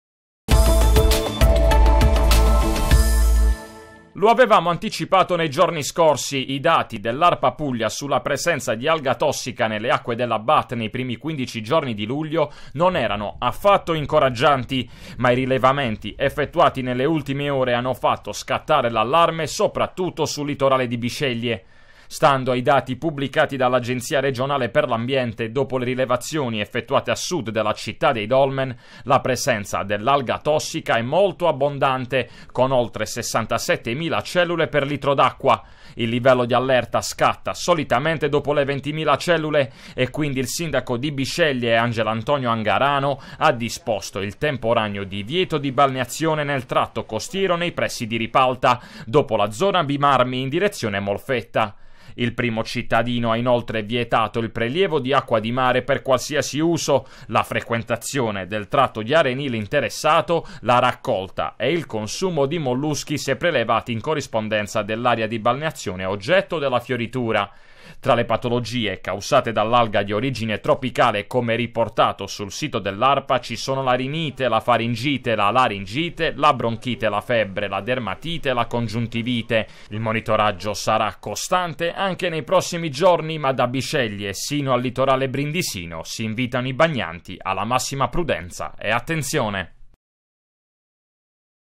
BAT | Allarme "alga tossica": scatta il divieto di balneazione a sud di Bisceglie
I rilevamenti degli ultimi giorni hanno evidenziato l'aumento esponenziale della "ostreopsis ovata"